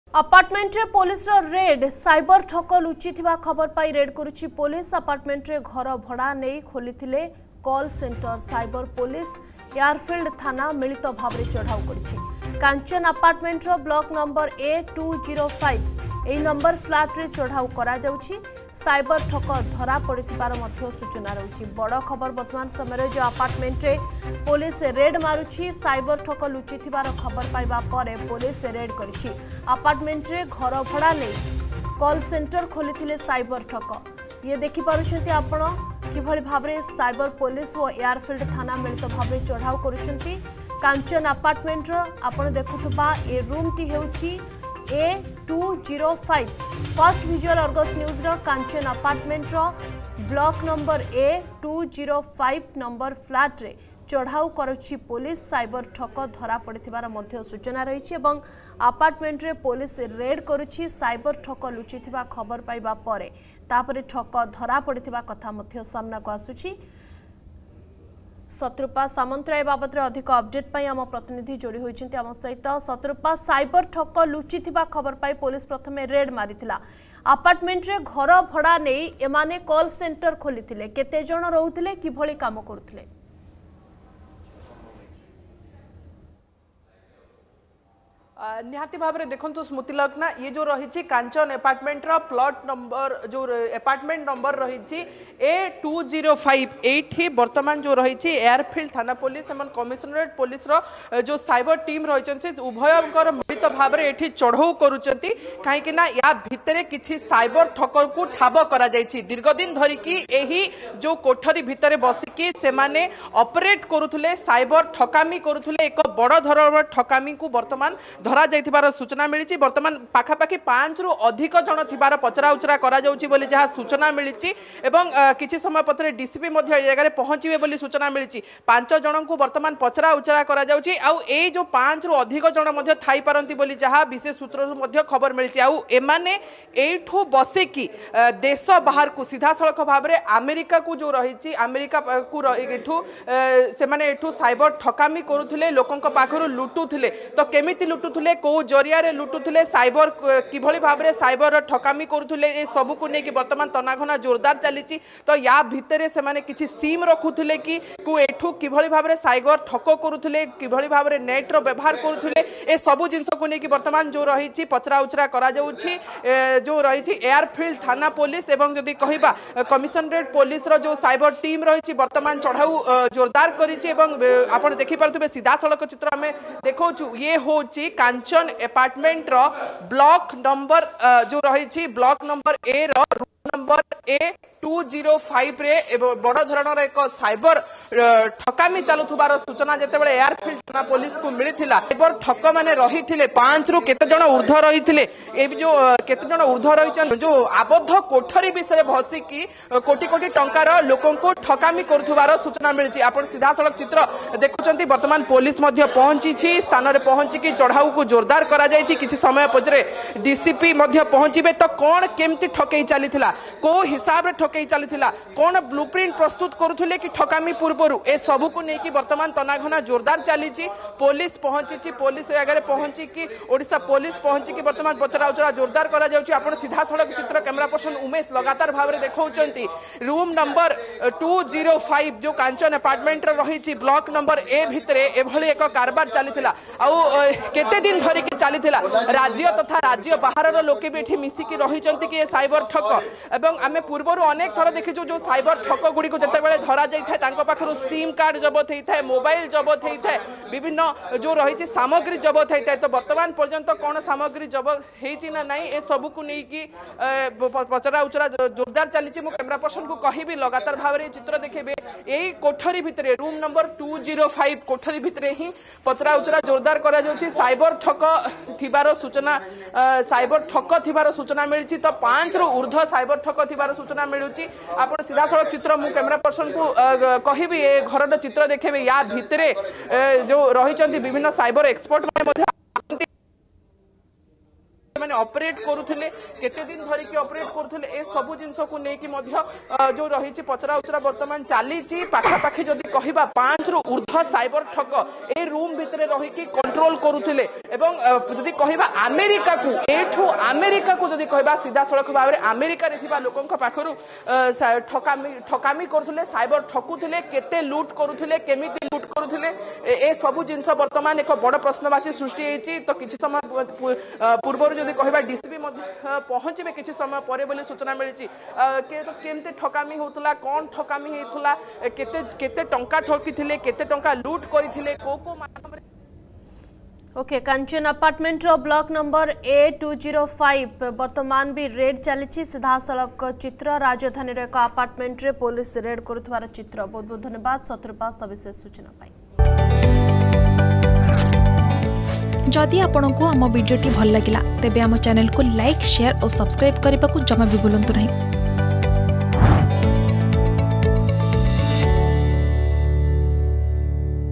ଆପାର୍ଟମେଣ୍ଟରେ ପୋଲିସର ରେଡ | cyber fraud | Bhubaneswar
ଆପାର୍ଟମେଣ୍ଟରେ ପୋଲିସର ରେଡ । ସାଇବର ଠକ ଲୁଚିଥିବା ନେଇ ରେଡ କରୁଛି ପୋଲିସ